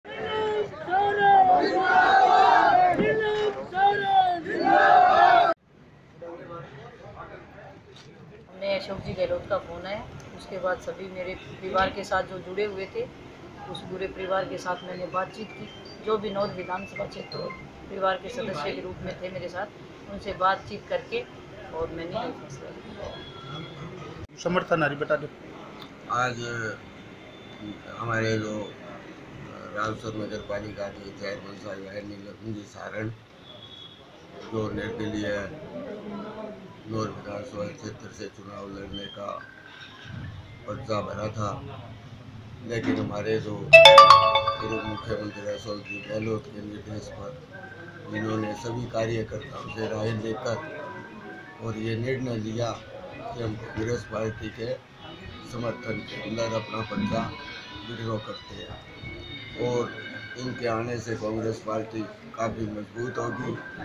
नोहर में कांग्रेस को बड़ी राहत, अशोक गहलोत ने इस बागी को मनाया
हनुमानगढ़. जिले की नोहर विधानसभा क्षेत्र से कांग्रेस प्रत्याशी अमित चाचाण को निर्दलीय प्रत्याशी नीलम सहारण ने अपना नामांकन वापस लेते हुए समर्थन दे दिया है. नीलम सहारण का कहना है कि अशोक गहलोत से बात होने के बाद उन्होंने यह फैसला लिया है.